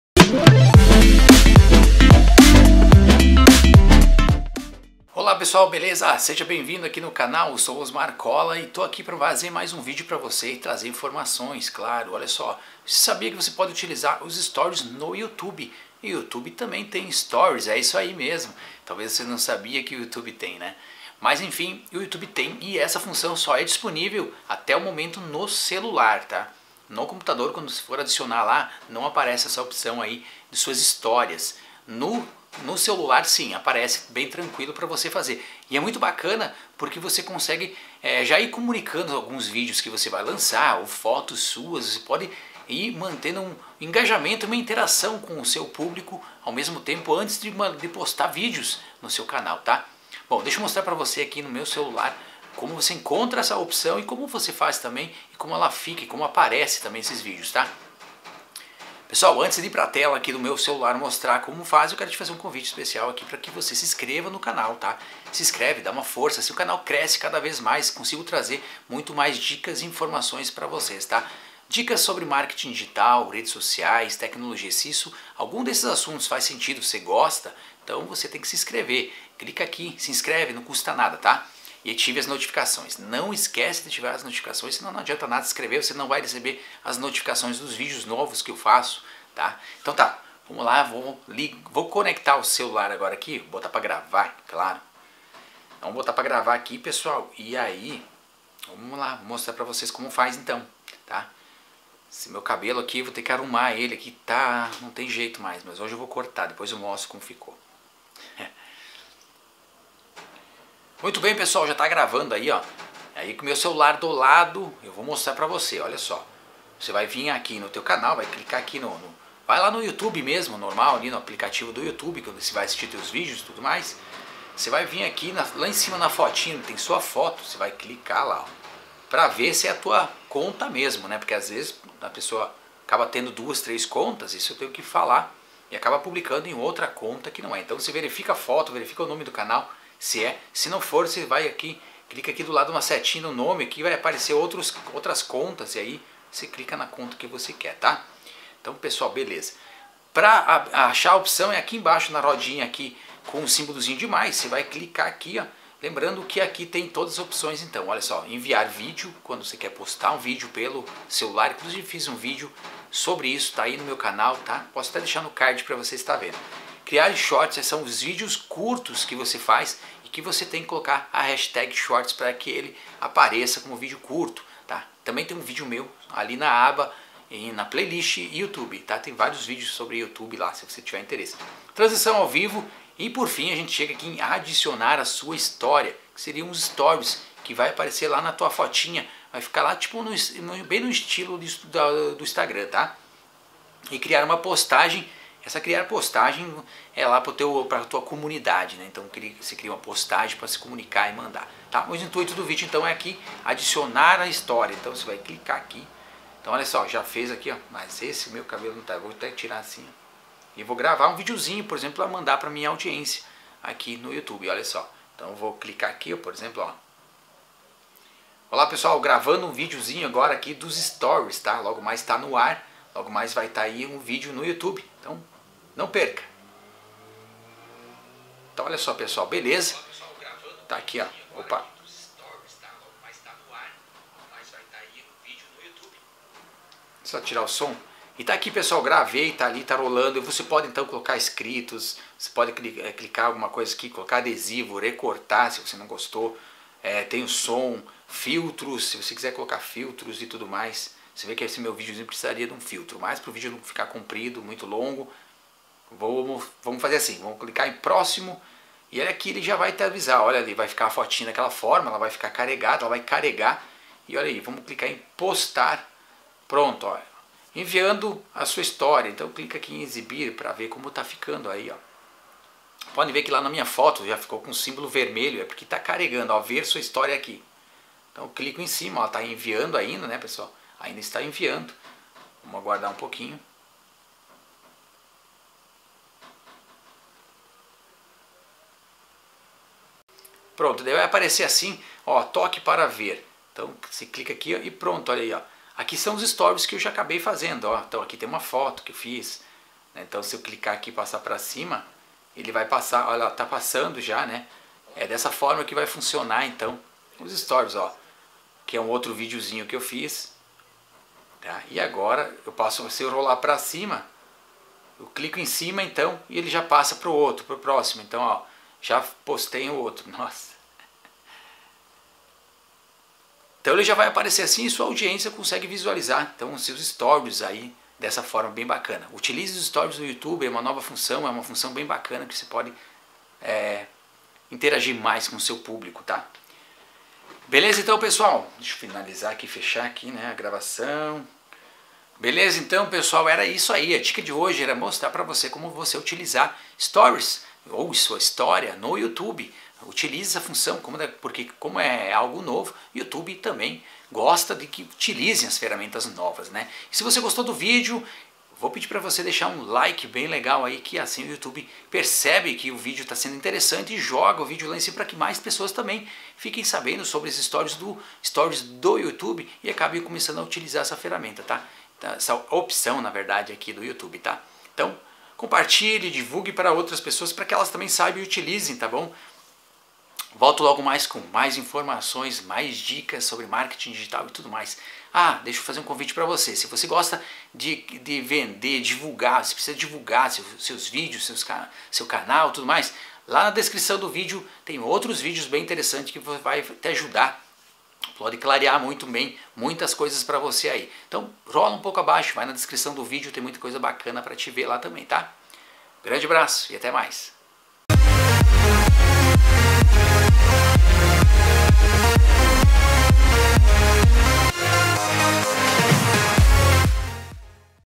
COMO USAR A FUNÇÃO HISTORIA DO YOUTUBE
como usar a função história do youtube - quer saber como ativar a aba histórias no seu canal do youtube pelo celular mas não tem ideia de como fazer isso? como ativar histórias no youtube pelo celular. no video de hoje irei te ensinar como ativar histórias no seu canal do youtube pelo celular.